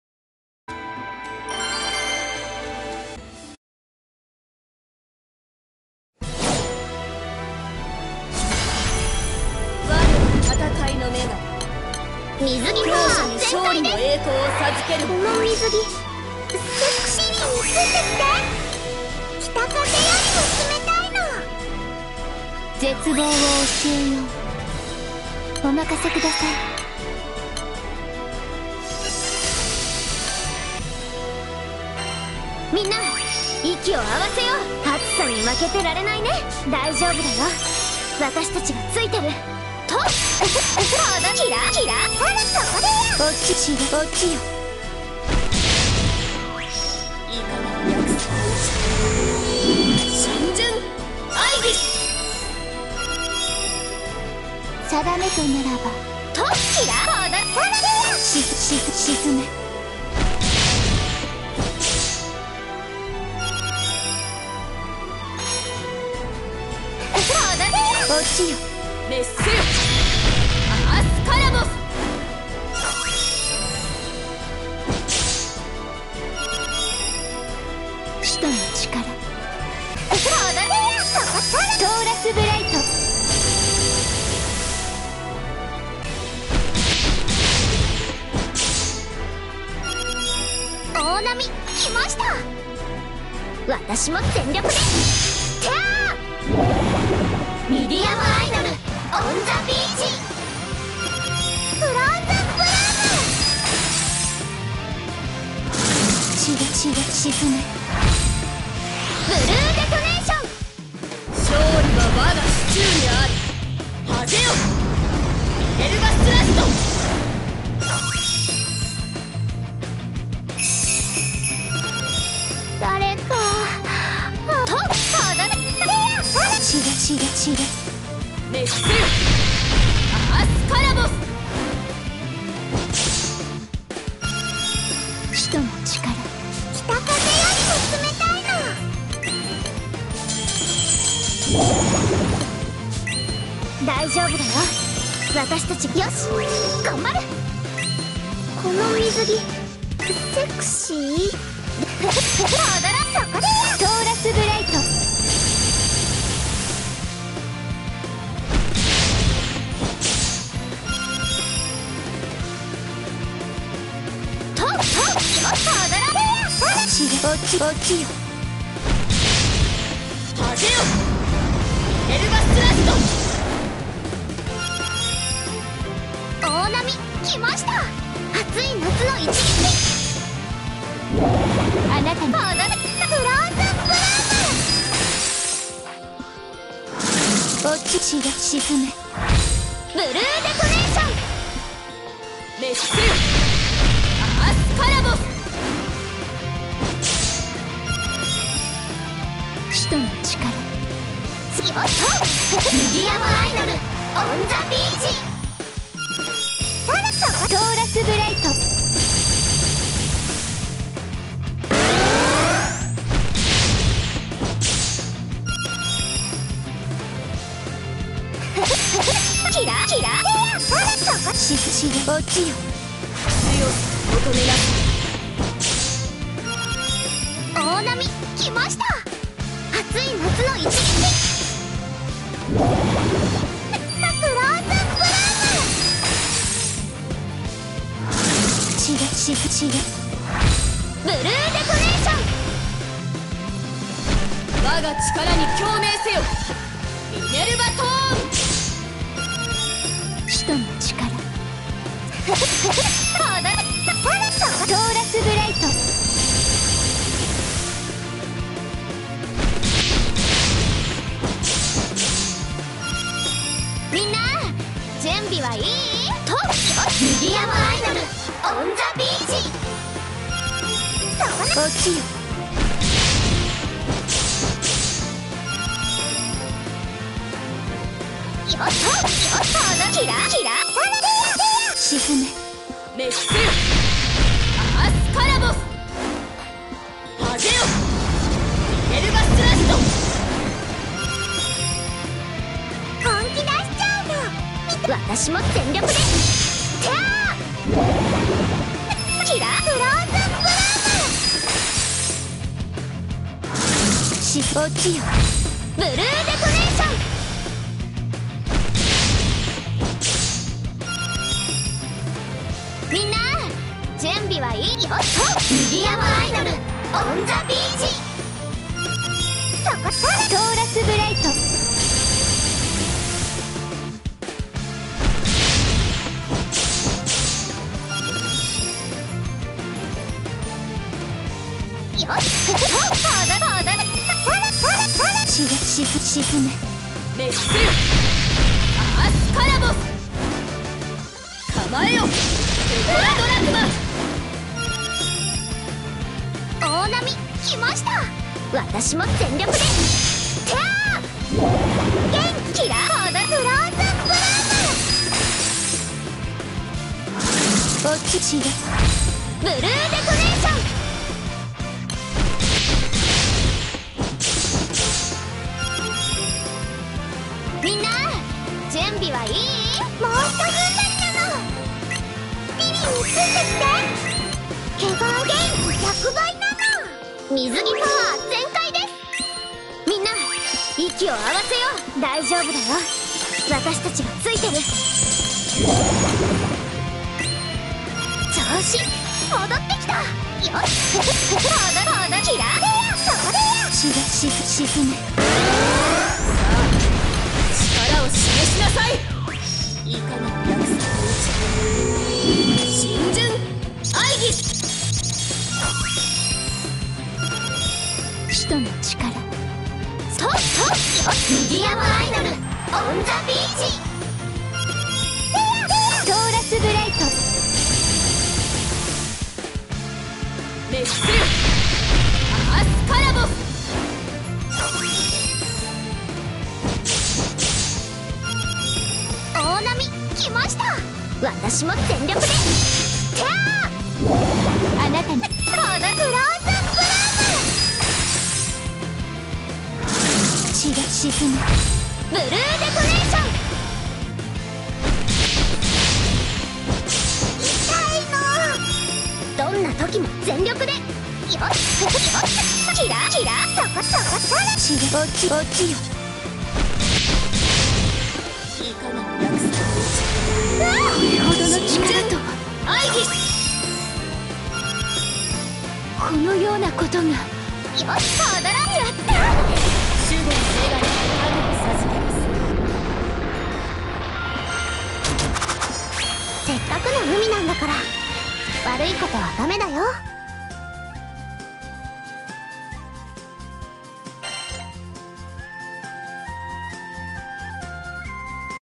SSR Summer Diantha 6/4 Rebalance Patch Showcase
Characters Used: Staff Girls All Day
Pool Used: Did auto to lower my damage so you can see diantha
ougi buff better